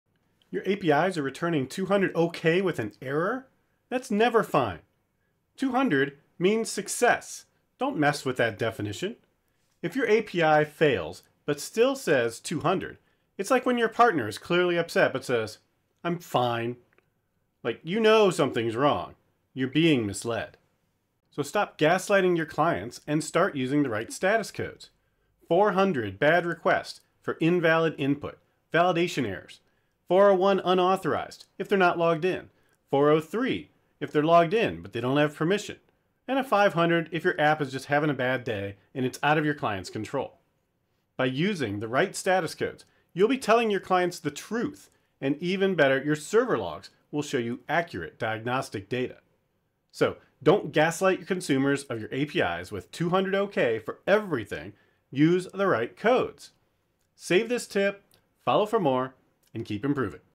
Stop Gaslighting your API Users
Use the right status code for your API responses! Don't just return 200 OK with an error - that's NEVER OK.